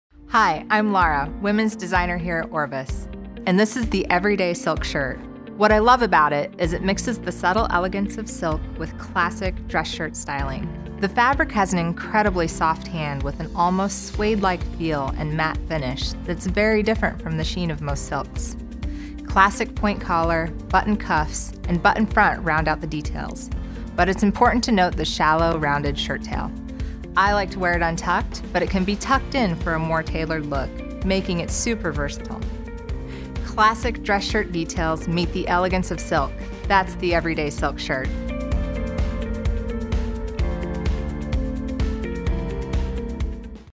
ORVIS - Everyday Silk Shirt
Hi, I’m Lara, Women’s Designer here at ORVIS, and this is the Everyday Silk Shirt. What I love about it is it mixes the subtle elegance of silk with classic, dress-shirt styling. The fabric has an incredibly soft hand with an almost suede-like feel and matte finish that’s very different from the sheen of most silks. Classic point collar, button cuffs, and button front round out the details, but it’s important to note the shallow, rounded shirttail. I like to wear it untucked, but it can be tucked in for a more tailored look, making it super versatile. Classic dress shirt details meet the elegance of silk, that’s the Everyday Silk Shirt.